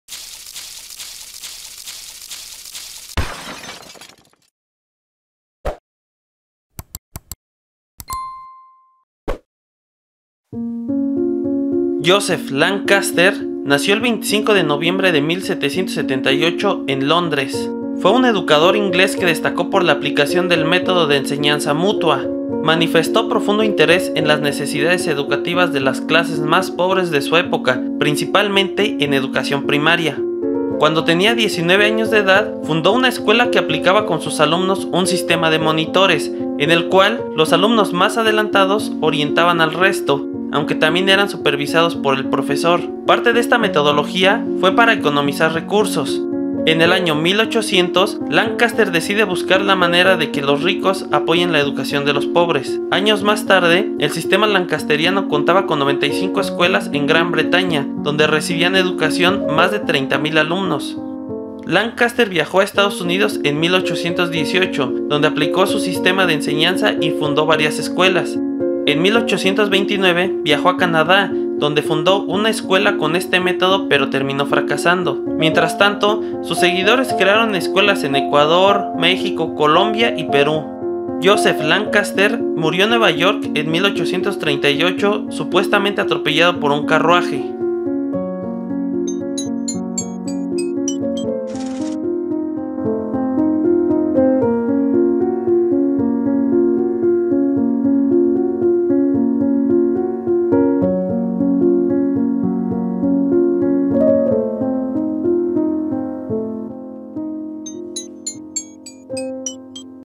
Biografía de Joseph Lancaster | Pedagogía MX
JOSEPH LANCASTER

Joseph Lancaster nació el 25 de noviembre de 1778 en Londres.

Fue un educador inglés que destacó por la aplicación del método de enseñanza mutua.

Manifestó profundo interés en las necesidades educativas de las clases más pobres de su época, principalmente en educación primaria.

Cuando tenía 19 años de edad fundó una escuela que aplicaba con sus alumnos un sistema de “monitores”, en el cual los alumnos más adelantados orientaban al resto, aunque también eran supervisados por el profesor.

Parte de esta metodología fue para economizar recursos.

En el año1800, Lancaster decide buscar la manera de que los ricos apoyen la educación de los pobres.

Años más tarde, el sistema Lancasteriano contaba con 95 escuelas en Gran Bretaña, donde recibían educación más de 30.000 alumnos.

Lancaster viajó a Estado Unidos en 1818, donde aplicó su sistema de enseñanza y fundó varias escuelas.

En 1829 viajó a Canadá, donde fundó una escuela con este método pero terminó fracasando.

Mientras tanto sus seguidores crearon escuelas en Ecuador, Colombia y Perú.

Joseph Lancaster murió en Nueva York en 1838 supuestamente atropellado por un carruaje.